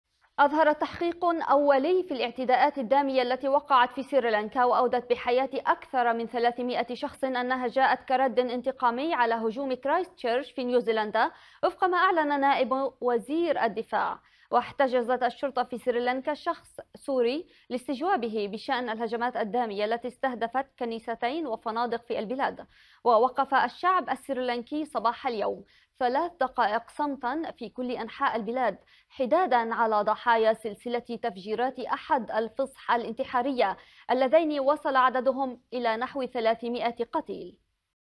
تفجيرات سريلانكا رد على هجوم نيوزيلندا
أخبار الآن | سريلانكا - أ ف ب

أظهر تحقيقُ أولي في الاعتداءاتِ الدامية التي وقعت في سريلانكا وأودت بحياةِ أكثرَ من ثلاثمئةِ شخص أنها جاءت "كردٍ انتقامي على هجومِ كرايست تشيرش" في نيوزيلندا، وفق ما أعلنَ نائبُ وزير الدفاع.

واحتجزت الشرطة في سريلانكا شخصا سورياً، لاستجوابه بشأنِ الهجمات الدامية التي استهدفت كنيسيتين وفنادق في البلاد.

ووقف الشعب السريلانكي صباح اليوم الثلاثاء، ثلاث دقائق صمتً في كلِ أنحاء البلاد حدادا على ضحايا سلسلةِ تفجيراتِ أحد الفصح الانتحارية الذين وصل عددهم الى نحو ثلاثمئةِ قتيل.

وقال وزير الدولة لشؤون الدفاع أمام البرلمان إن "التحقيقات الأولية كشفت أن ما حدث في سريلانكا كان رداً انتقامياً على الهجوم ضد المسلمين في كرايست تشيرش"، في إشارة إلى الهجوم الإرهابي الذي استهدف مسجدين في مدينة كرايست تشيرش بنيوزيلندا الشهر الماضي والذي راح ضحيته 50 قتيلاً .

و أضاف أن مجموعتين محليتين مسؤولتان عن الاعتداءات، بينهما جماعة "التوحيد الوطنية".

وكان متحدث باسم الشرطة السريلانكية أعلن في وقت سابق، الثلاثاء، ارتفاع حصيلة ضحايا التفجيرات إلى 310 قتلى. وقال المتحدث روان غوناسيكيرا في بيان إن السلطات استجوبت في هذه المرحلة 40 شخصاً على خلفية التفجيرات التي نسبت إلى جماعة محلية هي "جماعة التوحيد الوطنية".